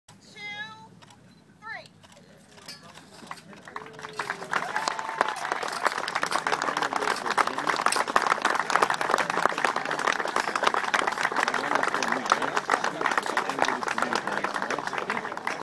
Groundbreaking for the Wickford Junction Commuter Rail Station - August 18, 2010.